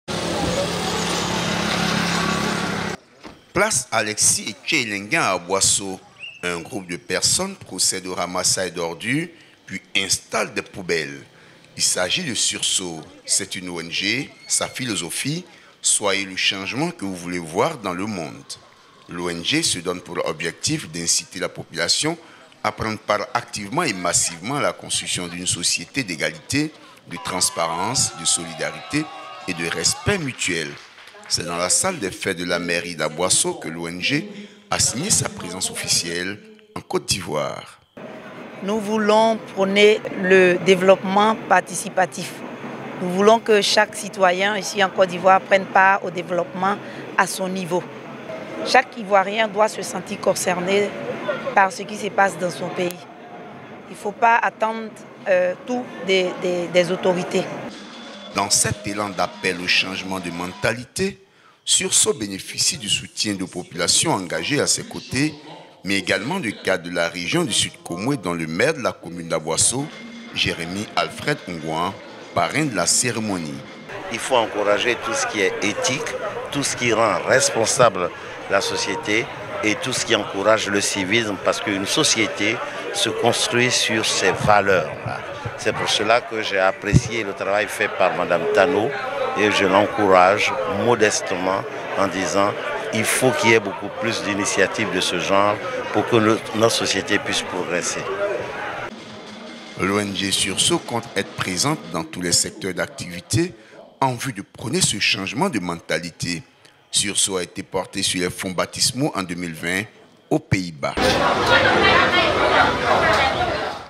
Région-Aboisso / L’ONG Sursaut s’installe officiellement à Aboisso
Place Alexis Etché Elleingand à Aboisso,  un groupe de personne procède aux ramassage d’ordures puis  installe des poubelles.  Il s’agit de Sursaut, C’est une ONG , sa philosophie « Soyez le changement que vous voulez voir dans le monde. »  L’Ong Se donne pour objectif d’inciter la population à prendre part activement et massivement à la construction d’une société d’égalité, de transparence, de solidarité et de respect mutuel.   C’est dans la salle des fêtes de la mairie d’Aboisso que l’ONG a signé sa présence officielle en Côte d’Ivoire.
L’ONG sursaut compte être présente dans tous les secteurs d’actvités en vue de prôner ce changement de mentalité. Sursaut a été porté sur les fonts baptismaux en 2020 au Pays Bas.